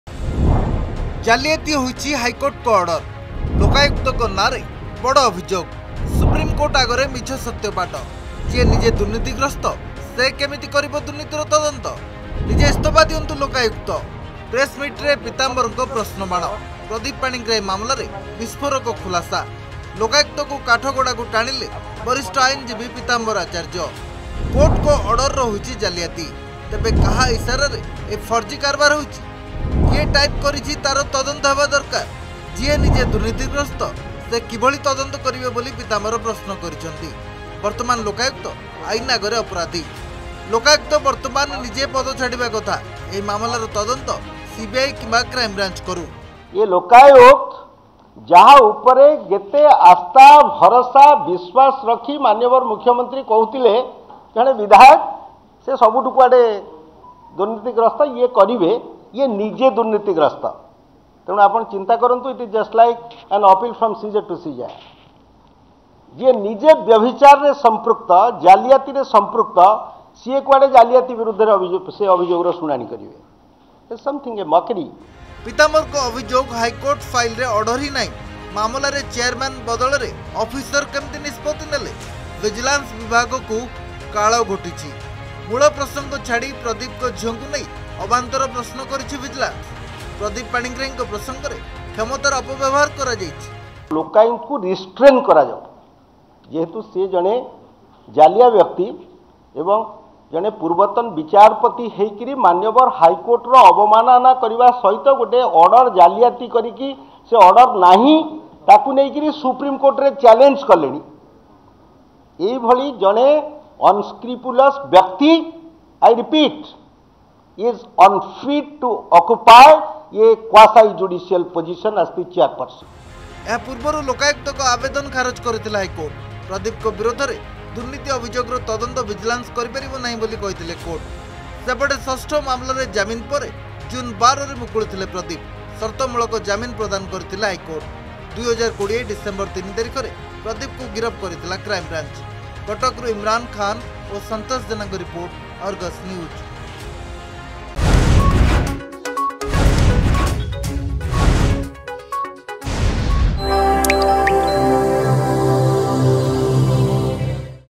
ପ୍ରଦୀପ ପାଣିଗ୍ରାହୀ ମାମଲାରେ ବଡ଼ ଅଭିଯୋଗ
ଲୋକାୟୁକ୍ତଙ୍କ ନାଁରେ ବଡ଼ ଅଭିଯୋଗ I ସୁପ୍ରିମକୋର୍ଟ ଆଗରେ ମିଛ ସତ୍ୟପାଠ I ଯିଏ ନିଜେ ଦୁର୍ନୀତିଗ୍ରସ୍ତ ସେ କେମିତି କରିବ ଦୁର୍ନୀତିର ତଦନ୍ତ? ଲୋକାୟୁକ୍ତଙ୍କୁ କାଠଗଡ଼ାକୁ ଟାଣିଲେ ବରିଷ୍ଠ ଆଇନଜୀବୀ ପୀତାମ୍ବର ଆଚାର୍ଯ୍ୟ ।